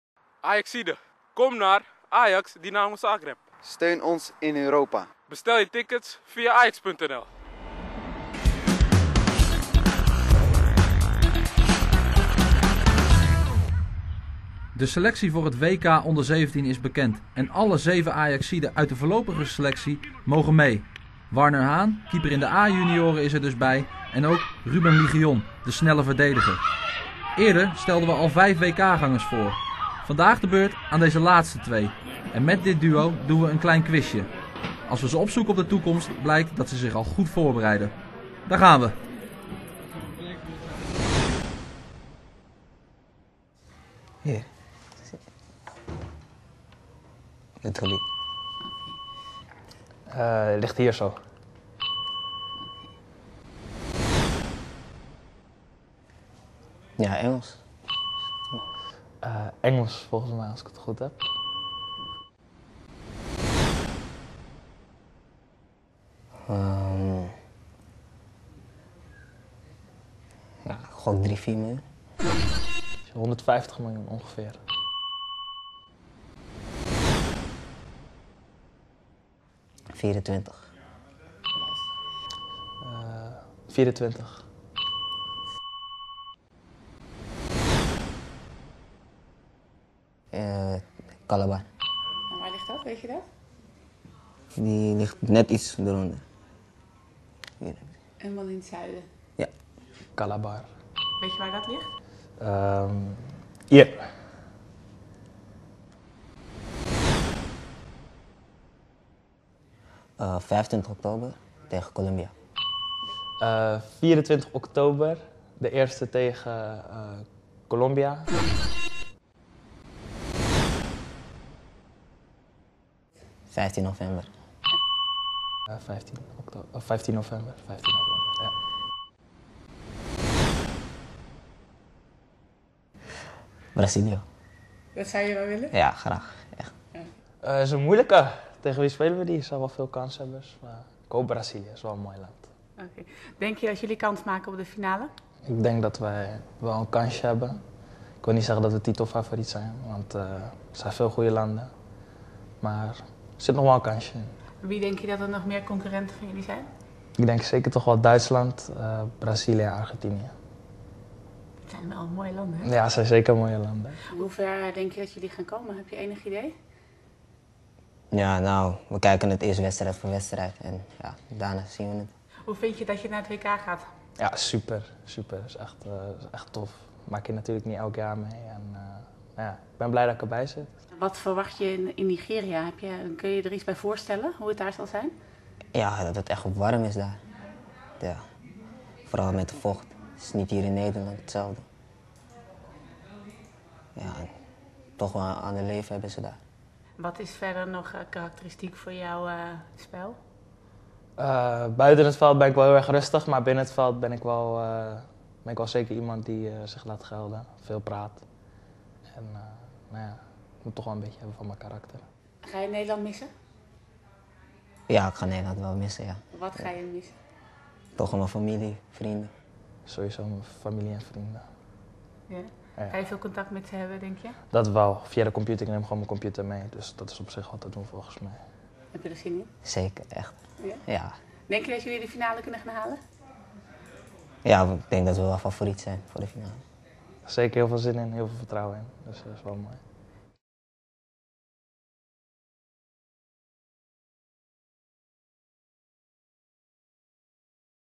Ajacieden geselecteerd voor WK-17
Maar liefst zeven Ajacieden zijn geselecteerd voor het jeugd-WK onder 17 jaar in Nigeria. We testen twee van hen, Ruben Ligeon en Warner Hahn, op hun kennis over het naderende toernooi.